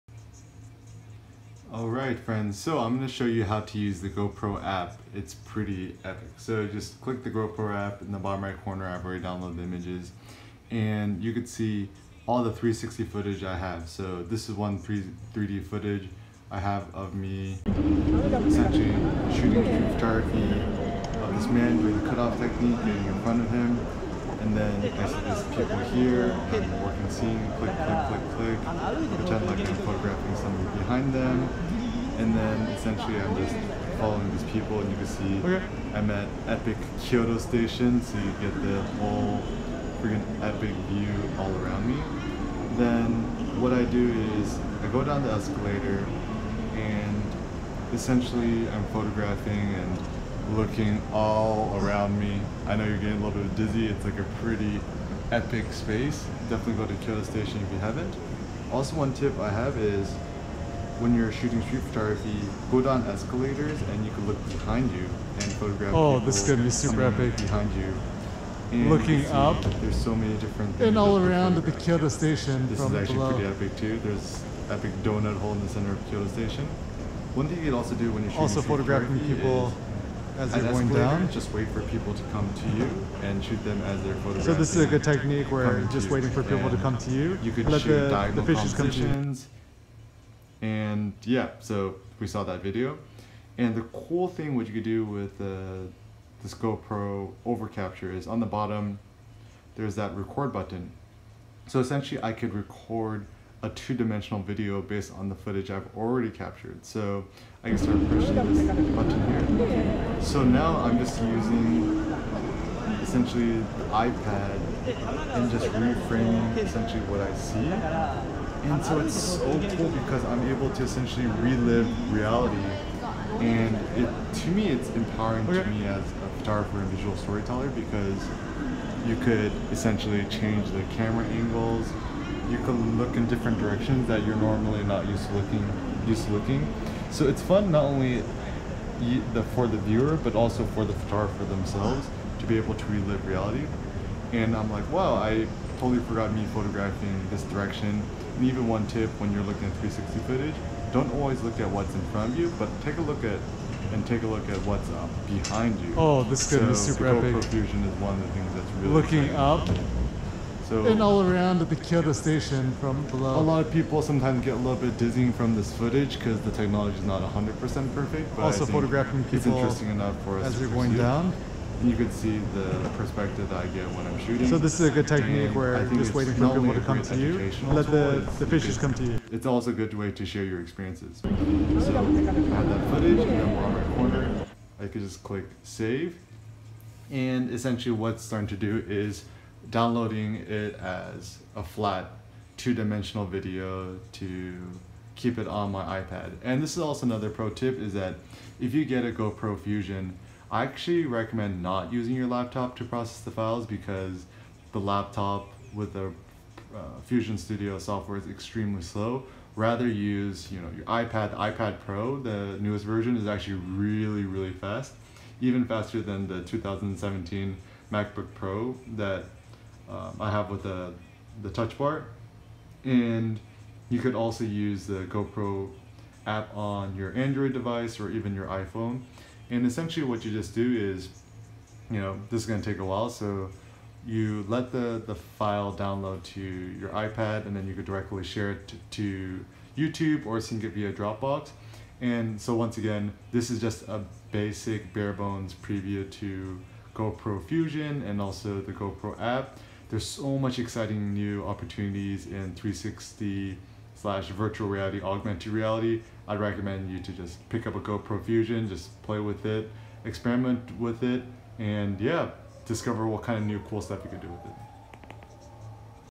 Introduction to GoPro Fusion Overcapture 360 Editing for Photographers and Visual Storytellers
Why the GoPro Fusion, Virtual Reality, Augmented Reality, and 360 is the Future of Photography